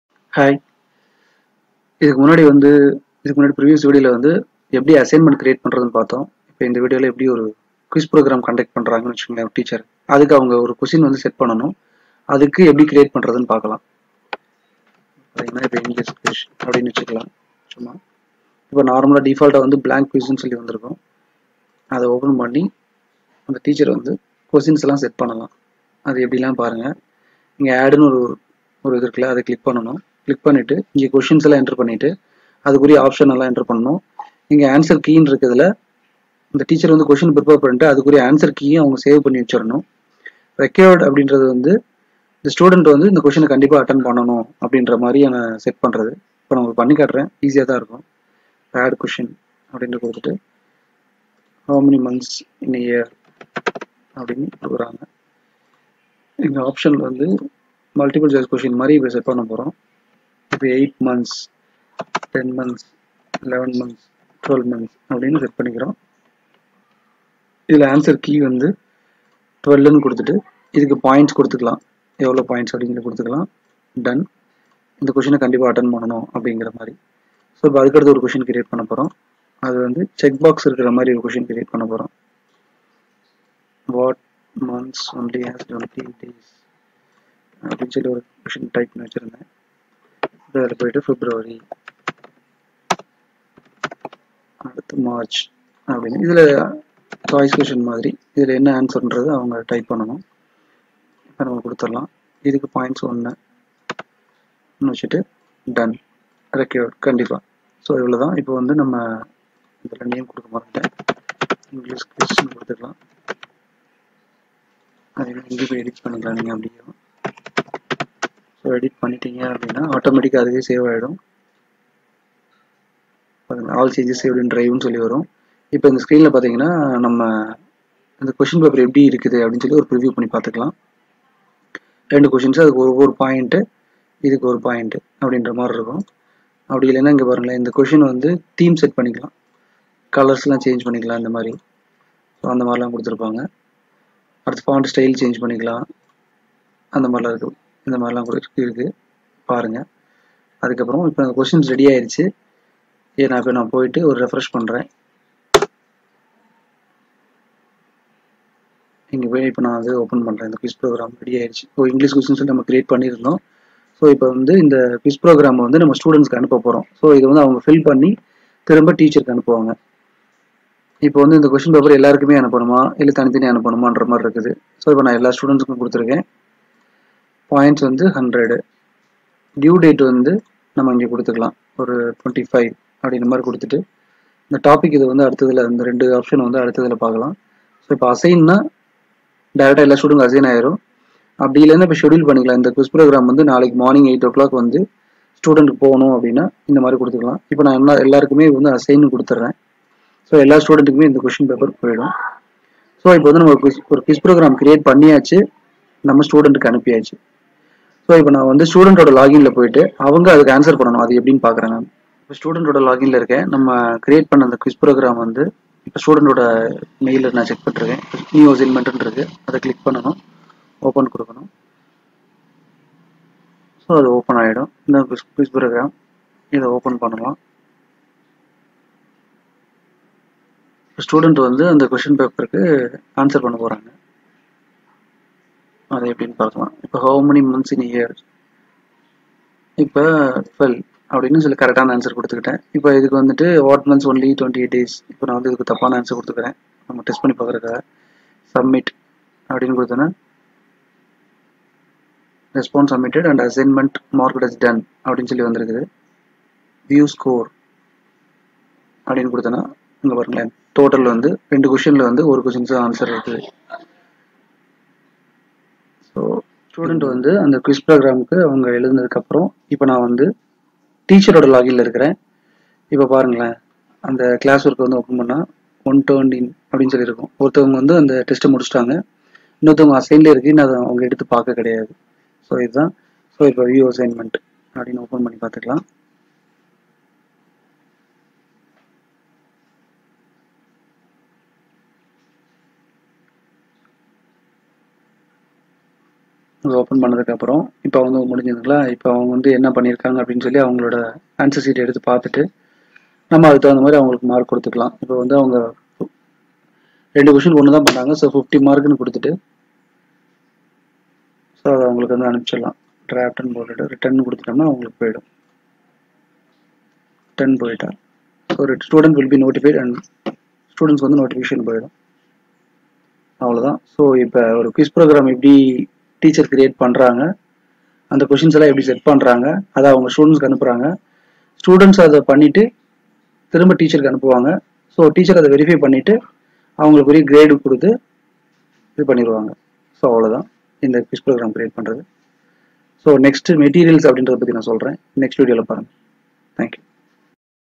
How to create a Quiz in Google Classroom in Tamil - Tutorial 4
How to create a Quiz in Google Classroom in Tamil
Creating a Google classroom explained step by step. Google classroom Converts a real time classroom into a virtual classroom.
Here a teacher can create a classroom and invites the students to join.Teachers can create an Assignment ( worksheet or homework) and share it with the students to complete it. and can create a Quiz assignments , Ask questions, Provide Materials , Evaluate the students progress and can conduct a group discussion .

Students can answer and complete their assignments and send back to teacher to review.